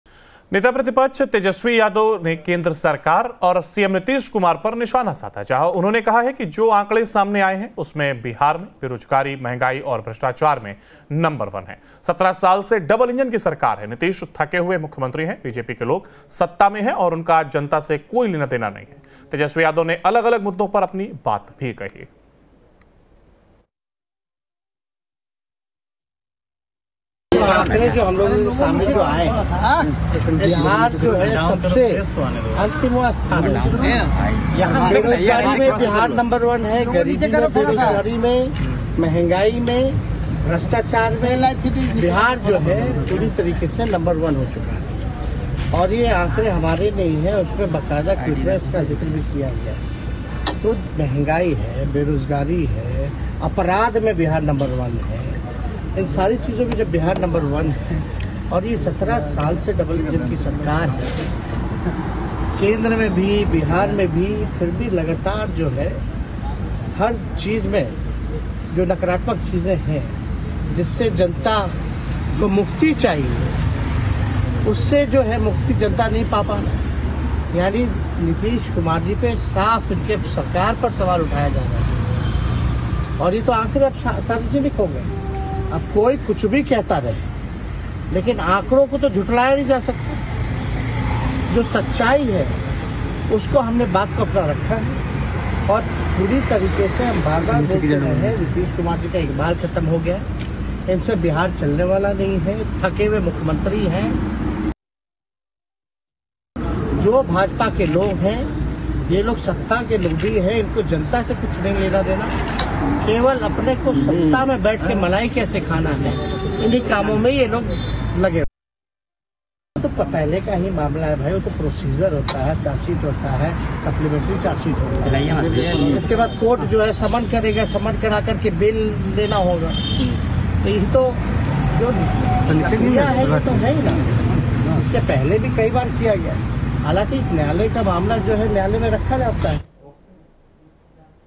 Bihar Politics : तेजस्वी ने नीतीश पर साधा निशाना, बिहार बेरोजगारी-अपराध-भ्रष्टाचार में 'नंबर वन'
Bihar Politics : तेजस्वी ने नीतीश पर साधा निशाना, बिहार बेरोजगारी-अपराध-भ्रष्टाचार में 'नंबर वन' | Bihar news | Nitish Kumar | Tejashwi Yadav | Bihar latest news

Airtel - 378
TATA Play - 1131
DARSH - 525
GTPL -268